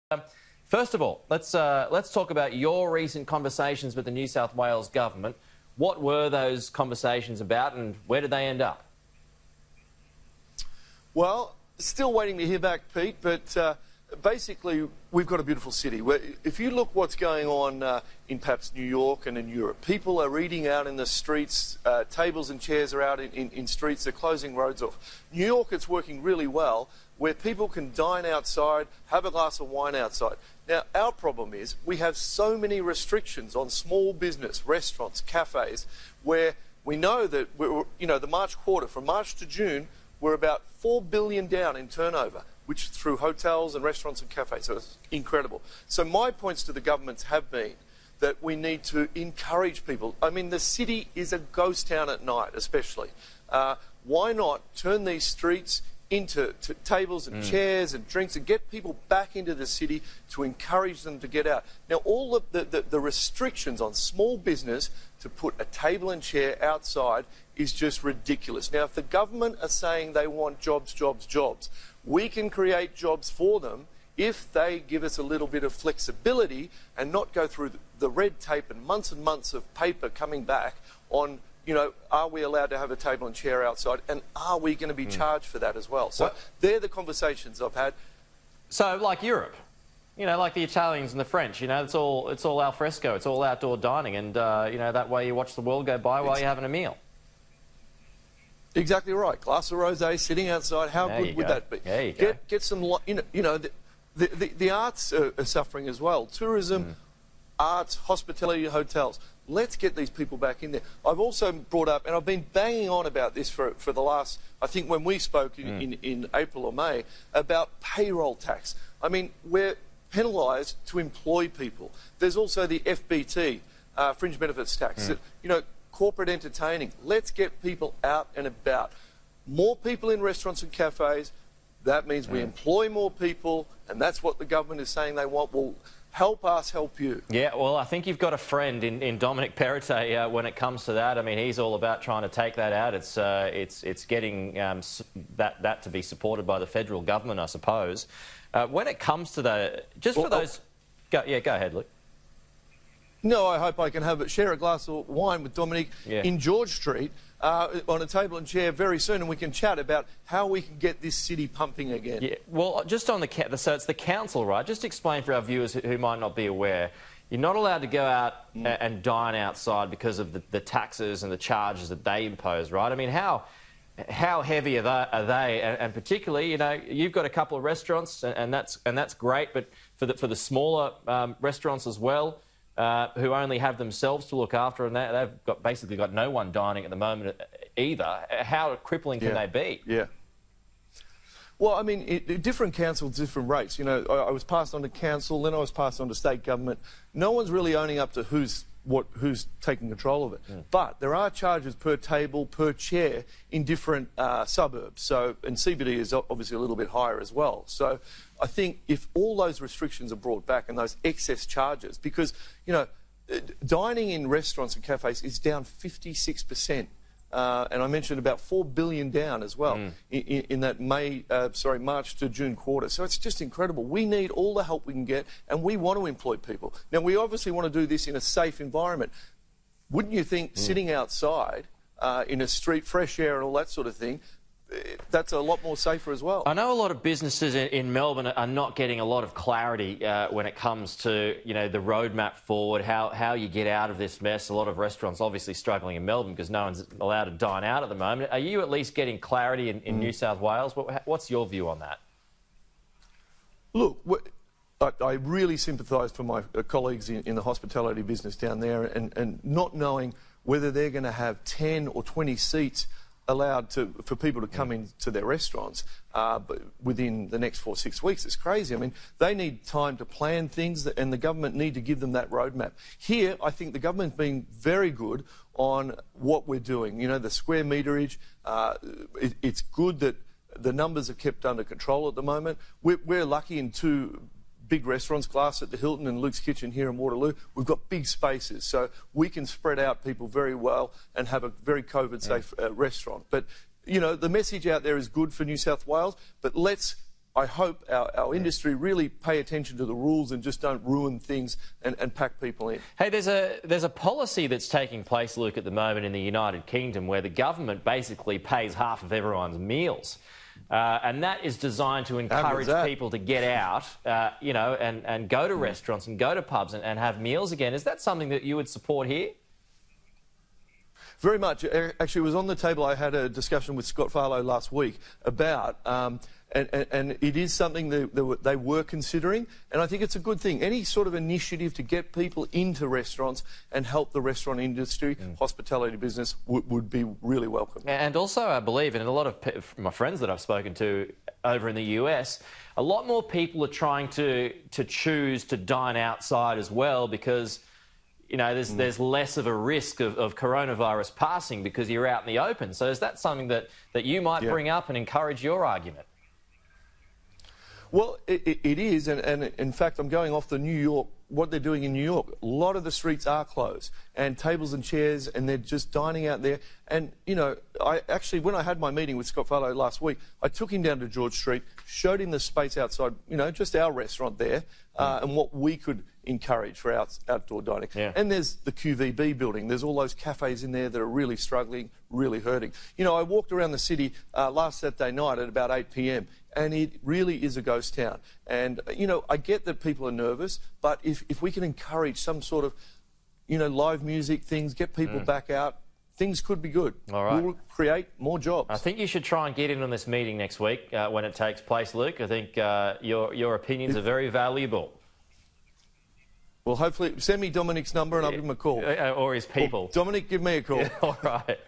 Cafe, restaurant owners urging government to cut 'red tape' around al fresco dining
Restaurateur and chef Luke Mangan is calling on the government to ease restrictions around al fresco dining options for small cafes and restaurants to ease operational costs as NSW businesses attempt to reopen.

Mr Mangan told Sky News the government needed to encourage people to start dining out.

“Let’s get people out and about, more people in restaurants and cafes that means we employ more people and that’s what the government is saying they want," he said.

"Help us, help you.

“The government is saying they want jobs, jobs, jobs, we can create jobs for them if they give us a little bit of flexibility and not go through the red tape and months and months of paper coming back on whether we're allowed to have a table and chair outside.”

Mr Mangan said additional costs for things such as putting tables and chairs on sidewalks made it harder for cafes and restaurants to begin viably reopening.

“We need all the help we can get, we want to employ people, we obviously want to do this in a safe environment," he said.

"Wouldn’t you think sitting outside in a street with fresh air and all that sort of thing, that’s a lot safer as well."

Image: AP